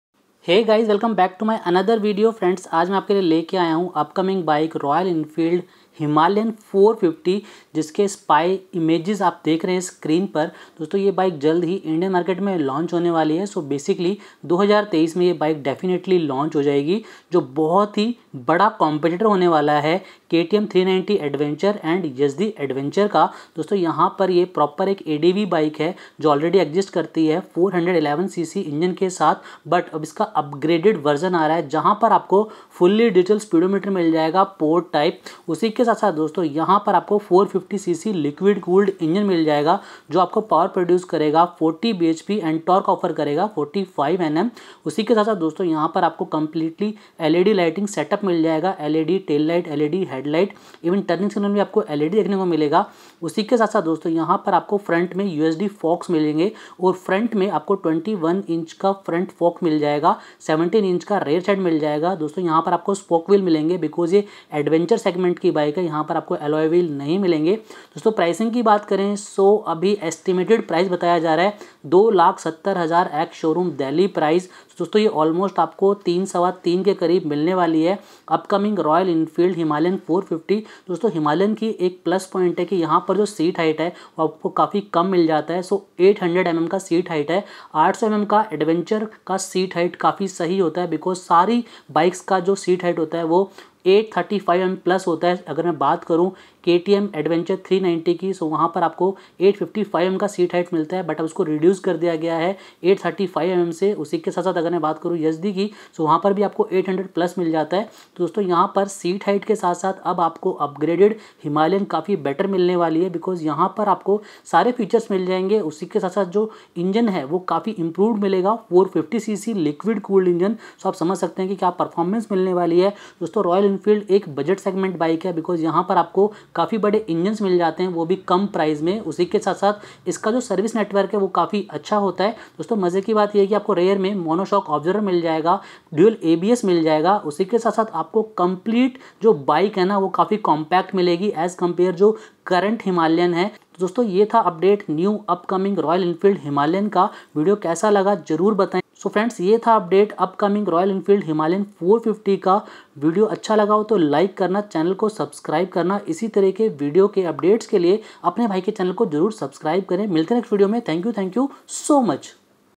Royal Enfield Himalayan 450 All Details Before Launch | Price | Features | Design | Smart Viker
Here is launch update of Royal Enfield Himalayan 450. All details of Royal Enfield Himalayan 450 like price, features, performance & comfort. Tough competitor of KTM 390 Adventure and Yezdi Adventure.